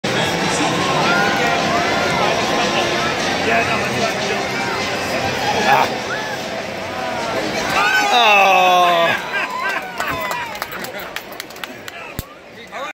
Aaron Judge throws a baseball to a kid
After snagging three baseballs in right field during Yankee batting practice, the kid calls out to Aaron Judge. Judge bounces it to him perfectly. Birthday celebration complete! Thanks, Aaron!